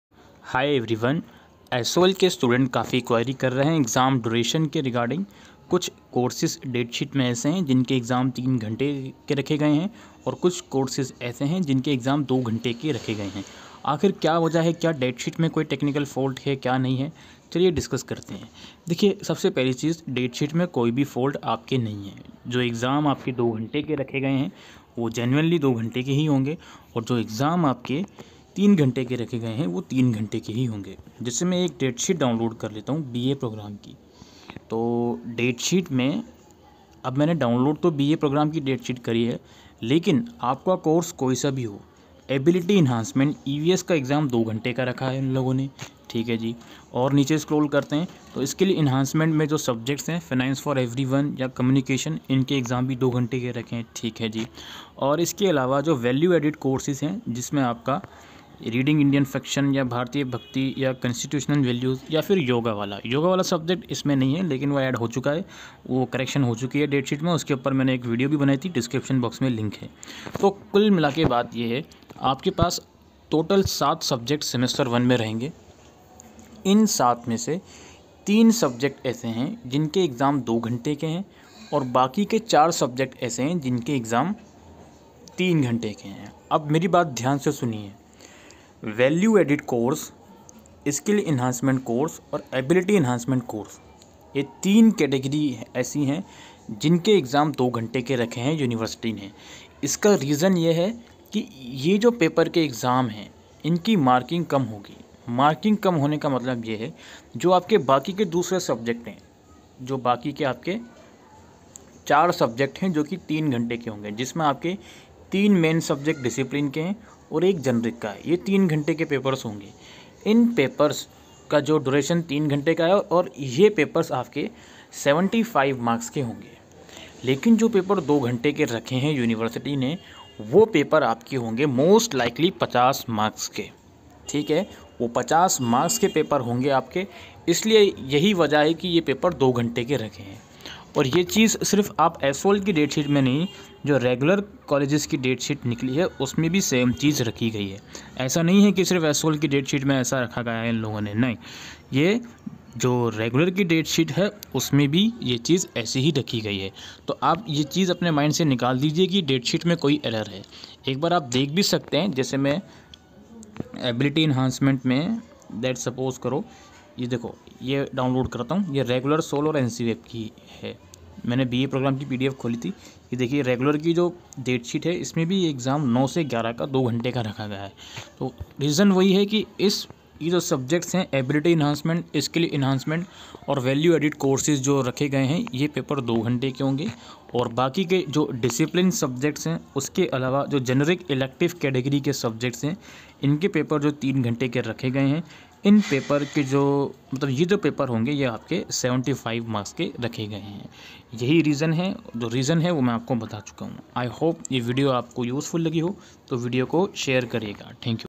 SOL first semester exam Duration कितनी हैं ? 2 or 3 hours | Ameeninfo
Hii Everyone
This Video Provides Information About

Exam Duration of various courses of Delhi university first'semester students
application to Regular Ncweb and sol students first semester March 2023